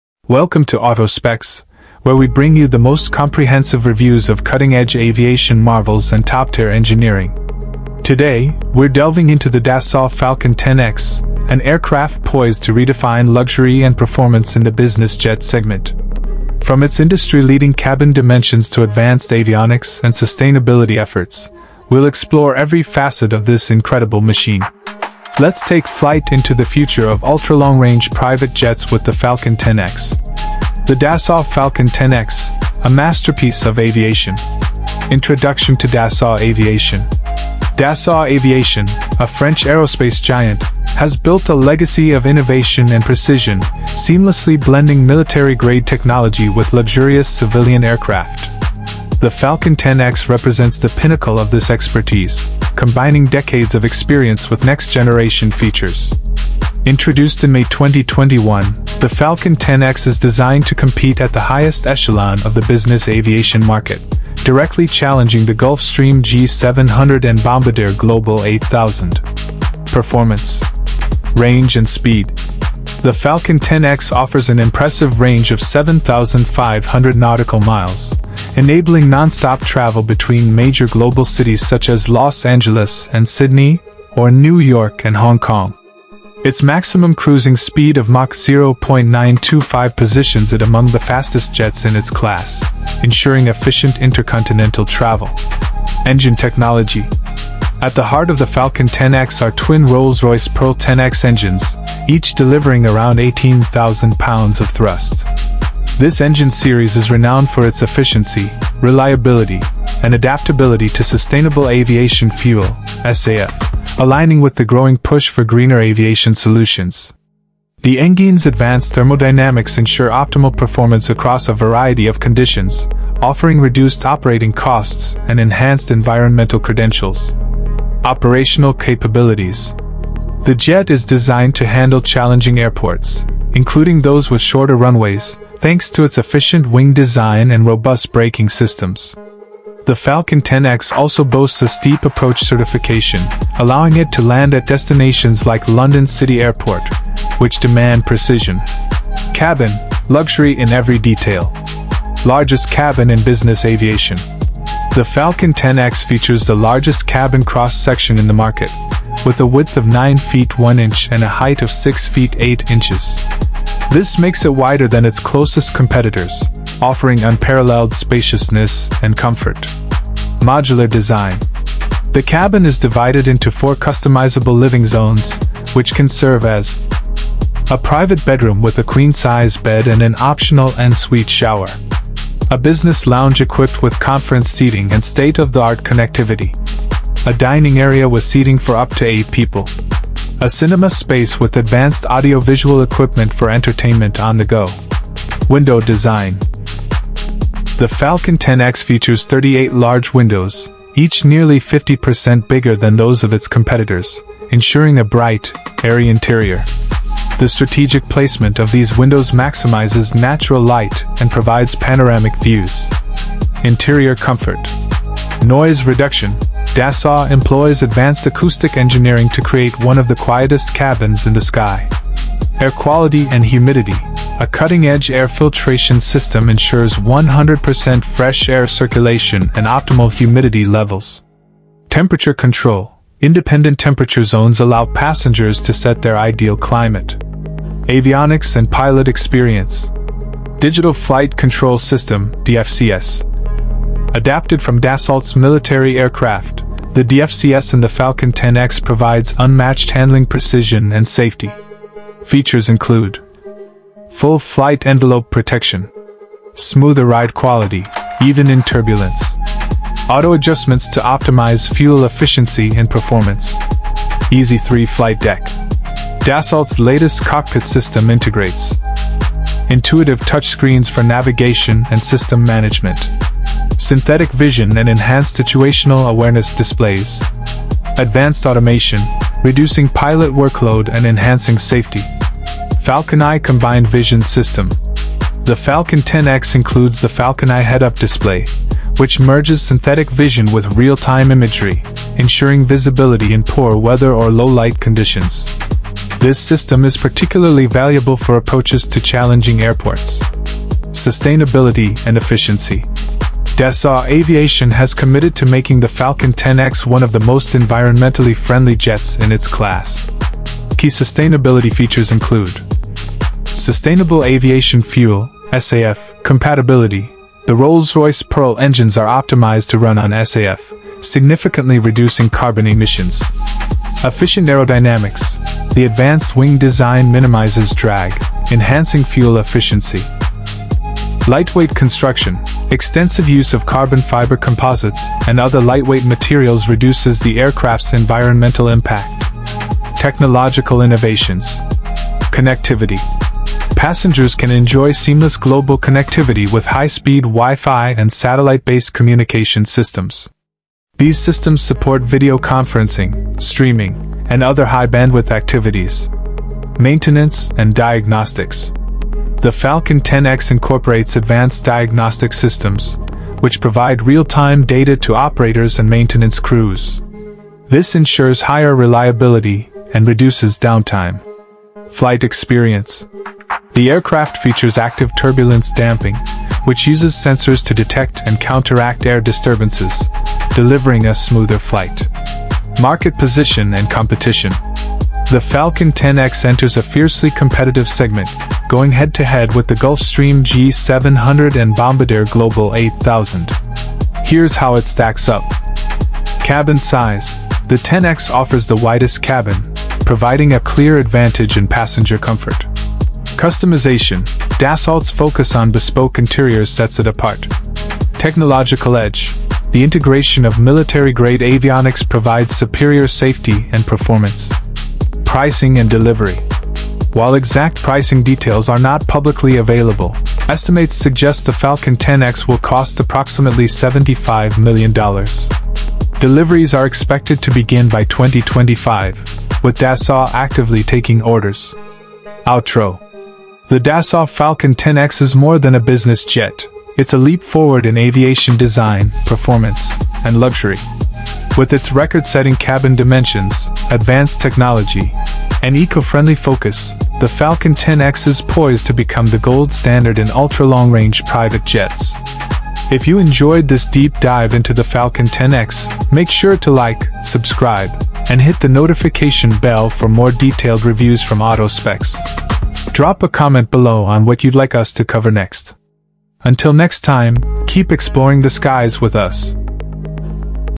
Why the Dassault Falcon 10X is a Game-Changer – Full Review | Inside Views | Auto Specs
Explore the ultimate in luxury and performance with our full review of the Dassault Falcon 10X, the pinnacle of business aviation. This comprehensive video takes you through every aspect of the Falcon 10X, including its unparalleled cabin design, cutting-edge avionics, and unmatched range of 7,500 nautical miles. Learn how its twin Rolls-Royce Pearl engines deliver power, efficiency, and sustainability, making it a leader in eco-friendly private jets. We dive into the jet's groundbreaking features like its four customizable living zones, advanced Digital Flight Control System (DFCS), and innovative FalconEye vision system.

Discover how the Falcon 10X sets new standards in luxury, with its industry-leading cabin size, oversized windows, and bespoke interior options. Whether you’re interested in its superior performance, state-of-the-art technology, or its role in the future of sustainable aviation, this video provides all the details you need.